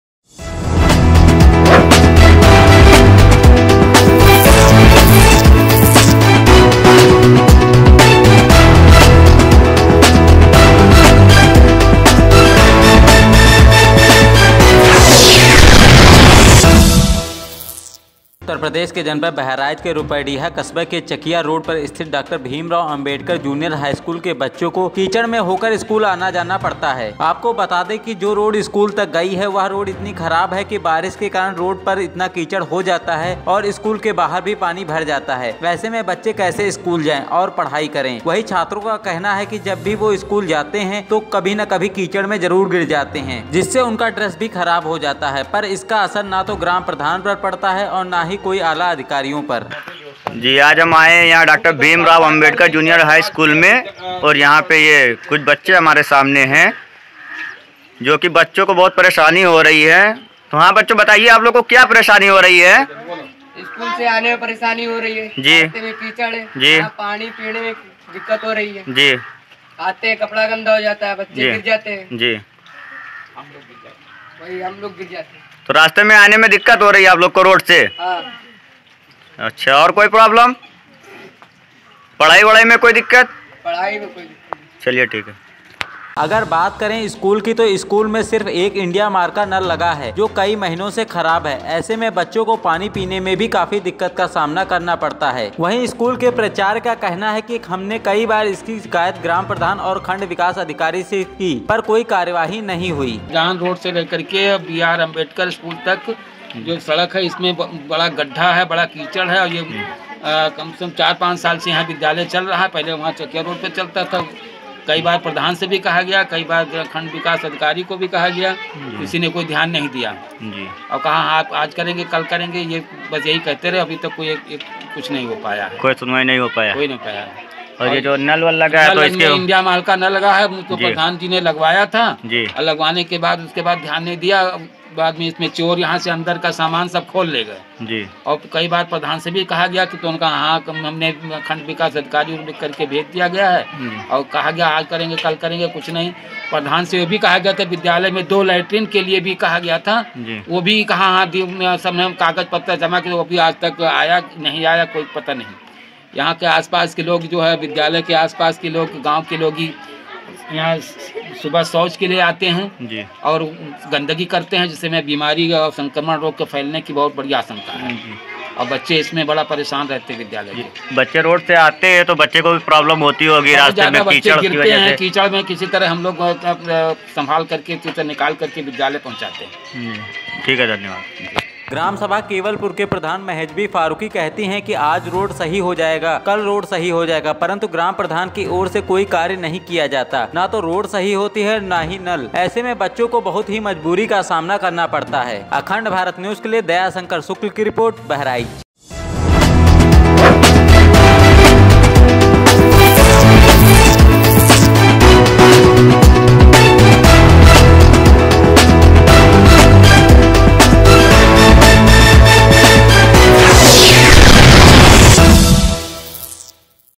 रोड सही न होने के कारण कीचड मे स्कूल जाने पर मजबूर है डॉ भीमराव अम्बेडकर स्कूल के छात्र...
दयाशंकर शुक्ल - बहराइच